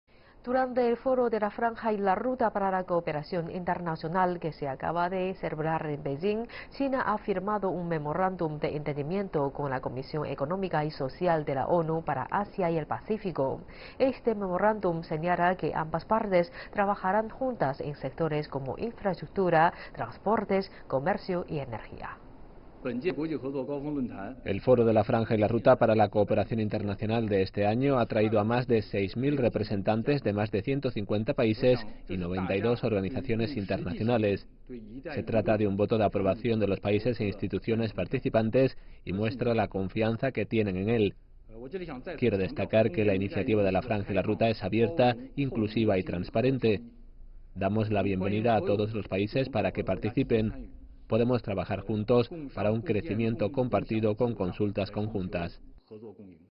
China y la Comisión de la ONU para Asia y el Pacífico firman un acuerdo económico
Durante el Foro de la Franja y la Ruta para la Cooperación Internacional que se acaba de celebrar en Beijing, China ha firmado un memorando de entendimiento con la Comisión Económica y Social de la ONU para Asia y el Pacífico. Este memorando señala que ambas partes trabajarán juntas en sectores como infraestructuras, transportes, comercio y energía.